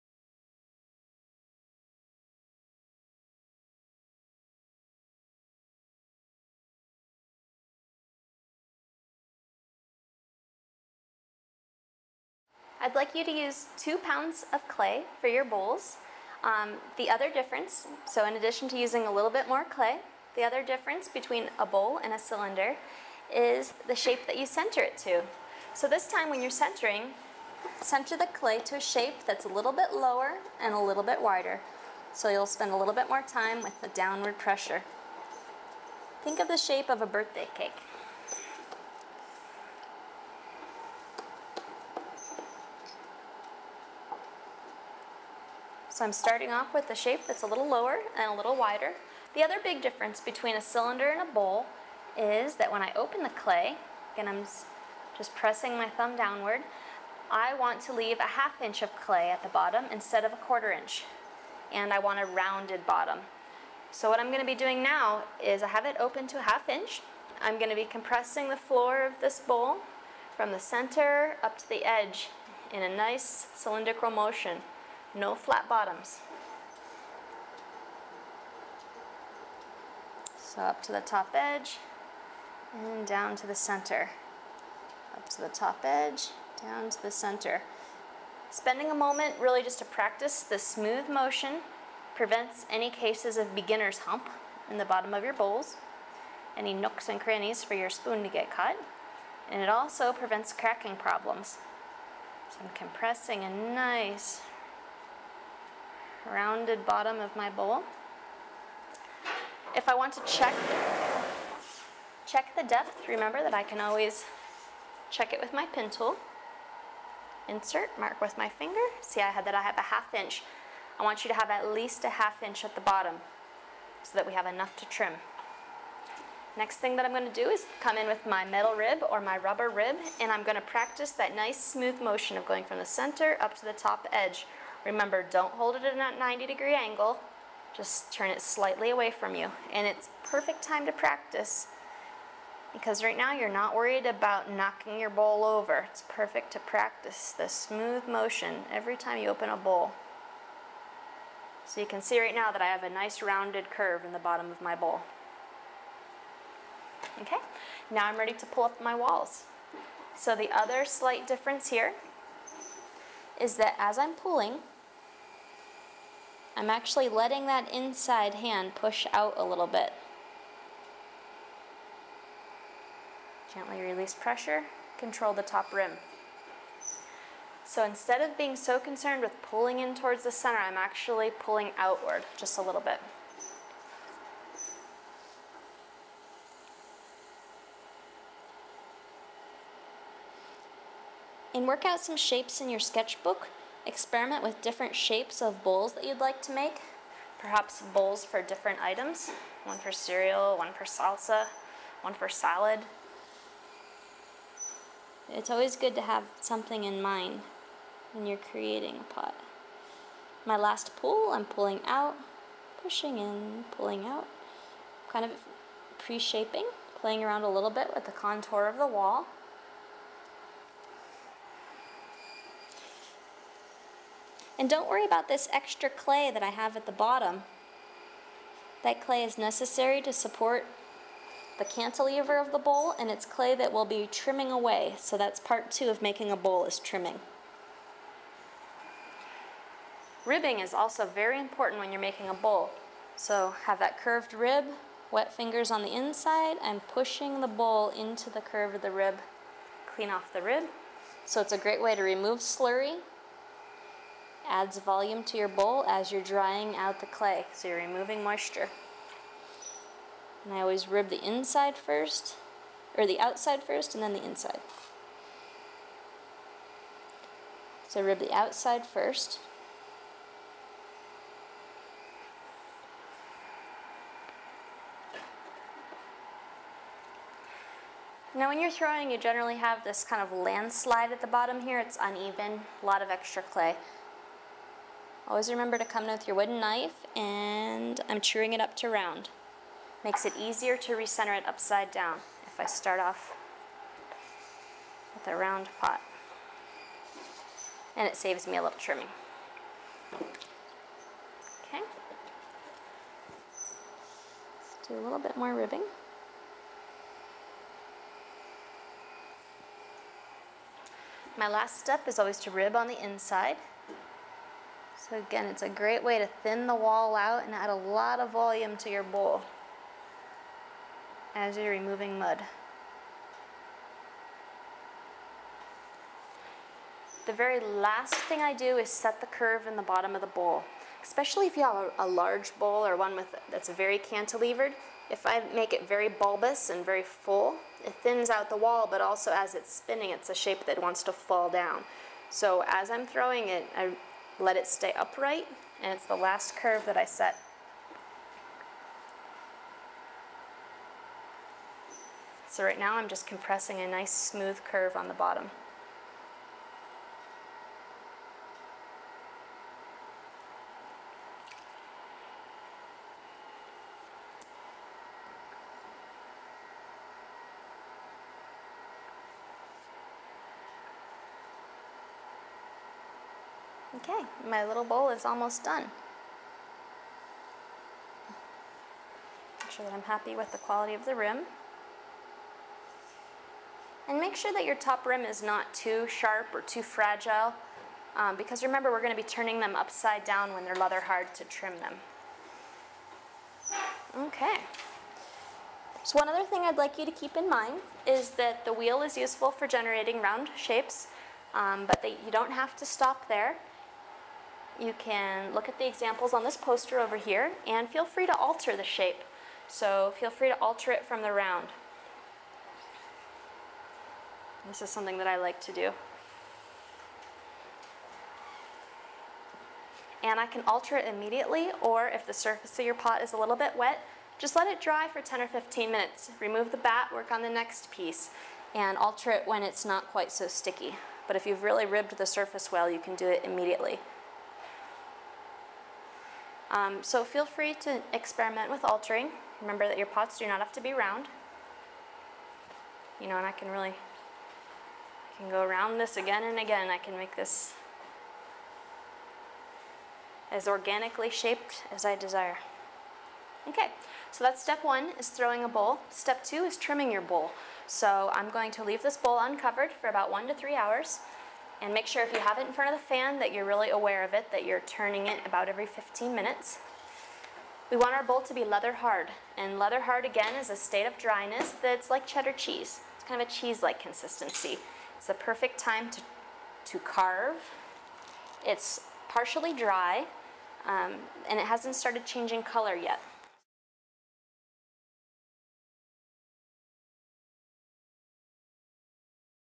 Claydemo 5: (Part A) Throwing a Bowl
Part Five of Twenty Part Series. Instructional Video showing basic bowl throwing techniques. Features: Lea Zoltowski, Assistant Professor, MSU Billings. DVD version available soon. Visit website for more videos and information.